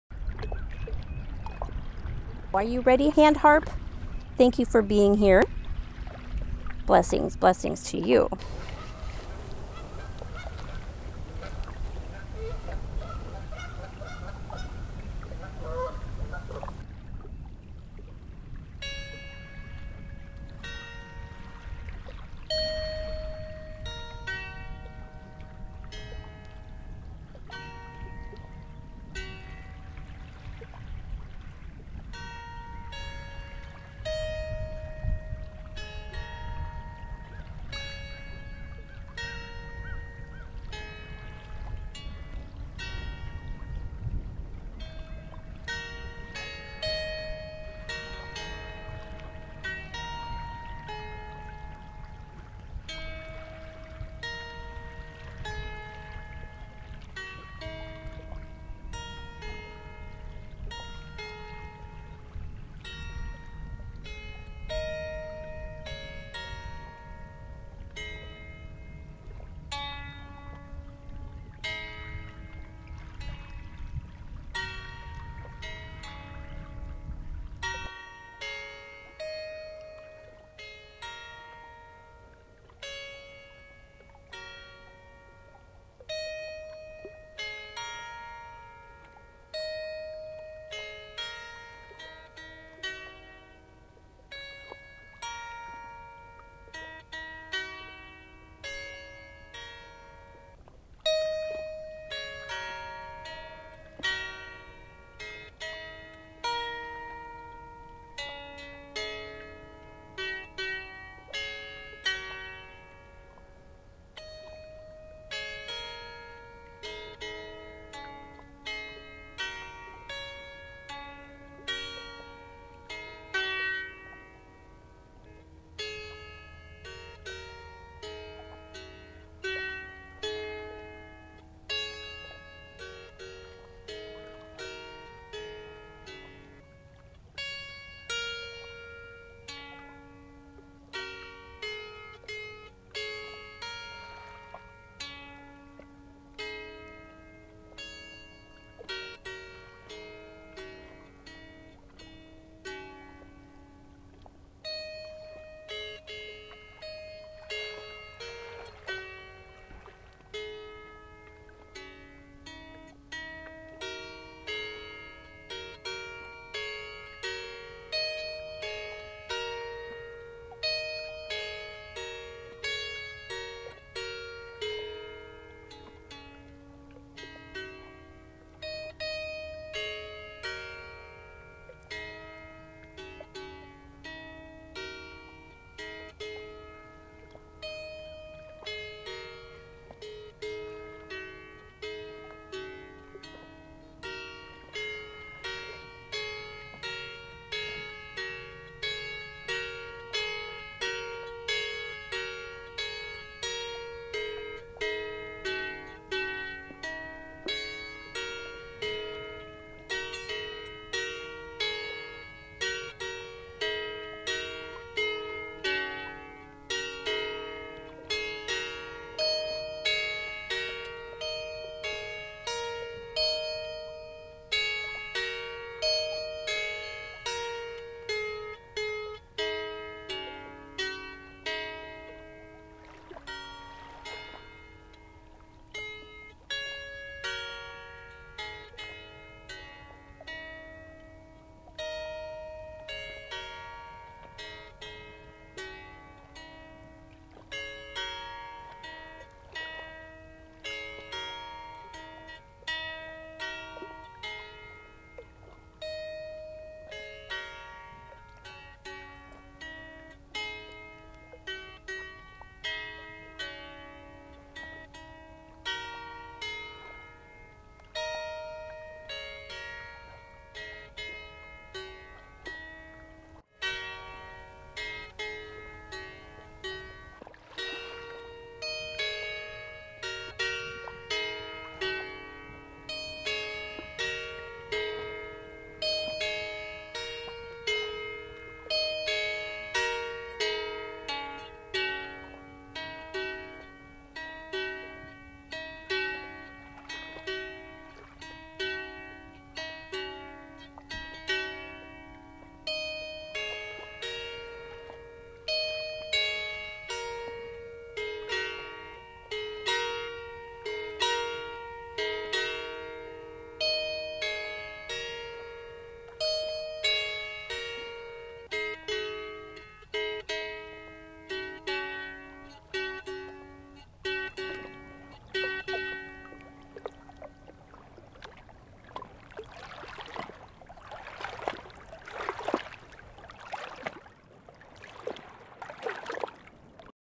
2022 04 Adlib Music Hand Harp New Moon
Participants of the New Moon Ceremony (2022-04 New Moon in Aries) requested the music be provided in a separate video.  They desire to listen to it throughout the New Moon phase.

We are honored and happy to provide.

Hand Harp Music
Divinely guided, spontaneously created.
The adlib format of these ceremonial songs allow Spirit to come through Lila and the instrument, delivering the energetic message that most optimally benefits the listener/participant.

Intention of this Song
We set an intention every New Moon and carry that intention through the entire moon cycle.  The intention of this moon cycle is:
  "To Prioritize Self.  Me First."
         Additionally, the energy of New Moon is to look into the Void and see all the opportunities relating to this intention that could be manifest throughout the remainder of the moon cycle.
         This is important because the music in this video is aligned with this intention and this moon phase.

May this music fill your heart with joy, and peace.  May it spark something deep within your soul.  May it bring you deep gratitude for Mother Earth, Madam Moon, and All That Is.

Ajo, Alleluh, Namaste.

Lila and the Crystal Weavers Gang.